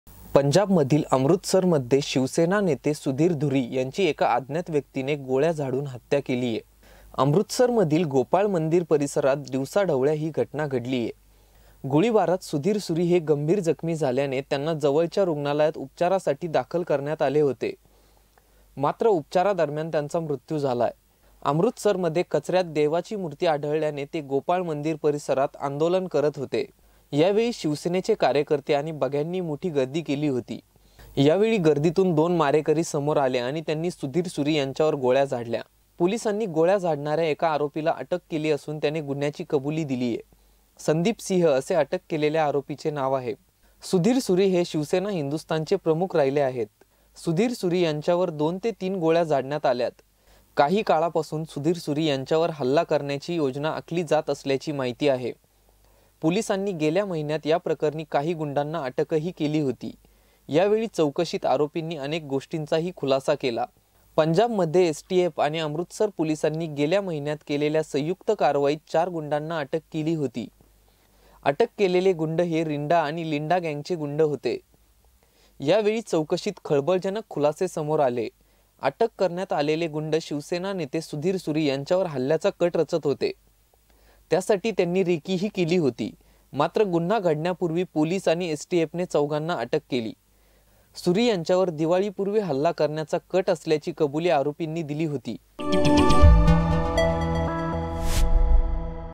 Sudhir Suri Murder Video | आंदोलनाला बसलेल्या शिवसेना नेत्याची हत्या करणाऱ्याला पोलिसांकडून अटक
पंजाबमधील अमृतसरमध्ये शिवसेना नेते सुधीर सूरी यांची एका अज्ञात व्यक्तीने गोळ्या झाडून हत्या केलीये...अमृतसरमधील गोपाळ मंदिर परिसरात दिवसाढवळ्या ही घटना घडलीये...गोळीबारात सुधीर सूरी गंभीर जखमी झाल्याने त्यांना जवळच्या रुग्णालयात उपचारासाठी दाखल करण्यात आले होते...मात्र उपचारादरम्यान त्यांचा मृत्यू झालाय...अमृतसरमध्ये कचऱ्यात देवाची मूर्ती आढळल्याने ते गोपाळ मंदिर परिसरात आंदोलन करत होते...यावेळी शिवसेनेचे कार्यकर्ते आणि बघ्यांनी मोठी गर्दी केली होती...यावेळी गर्दीतून २ मारेकरी समोर आले आणि त्यांनी सुधीर सूरी यांच्यावर गोळ्या झाडल्या...पोलिसांनी गोळी झाडणाऱ्या एका आरोपीला अटक केली असून त्याने गुन्ह्याची कबुली दिलीये...संदीप सिंह असे अटक केलेल्या आरोपीचे नाव आहे...सुधीर सूरी हे शिवसेना हिंदुस्थानचे प्रमुख राहिले आहेत...सुधीर सूरी यांच्यावर २ ते ३ गोळ्या झाडण्यात आल्यात...काही काळापासून सुधीर सूरी यांच्यावर हल्ला करण्याची योजना आखली जात असल्याची माहिती आहे...पोलिसांनी गेल्या महिन्यात याप्रकरणी काही गुंडांना अटकही केली होती...यावेळी चौकशीत आरोपींनी अनेक गोष्टींचाही खुलासा केला...पंजाबमध्ये एसटीएफ आणि अमृतसर पोलिसांनी गेल्या महिन्यात केलेल्या संयुक्त कारवाईत ४ गुंडांना अटक केली होती...अटक केलेले गुंड हे रिंडा आणि लिंडा गॅंगचे गुंड होते...यावेळी चौकशीत खळबळजनक खुलासे समोर आले...अटक करण्यात आलेले गुंड शिवसेना नेते सुधीर सूरी यांच्यावर हल्ल्याचा कट रचत होते...त्यासाठी त्यांनी रेकीही केली होती...मात्र गुन्हा घडण्यापूर्वी पोलीस आणि एसटीएफने चौघांना अटक केली...सूरी यांच्यावर दिवाळीपूर्वी हल्ला करण्याची कबुली आरोपींनी दिली होती...